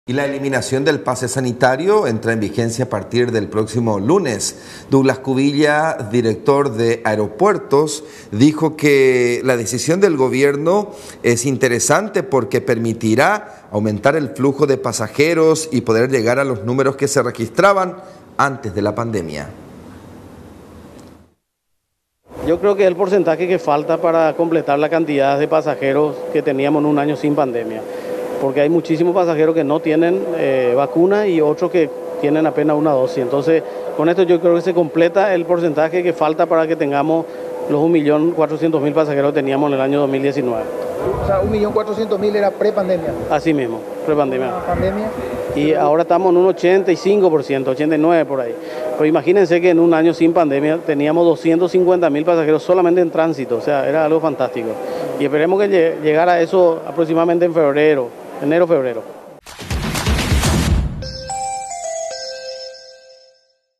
Eliminación de exigencia de vacunación
Con la eliminación de la exigencia del carnet de vacunación anti covid para ingresar al país, que entrará a regir desde el próximo lunes, el director del Aeropuerto Internacional Silvio Pettirossi, Douglas Cubilla, celebró la decisión del gobierno argumentando que esto permitirá aumentar el flujo de pasajeros y poder llegar a los números que se registraban antes de la pandemia.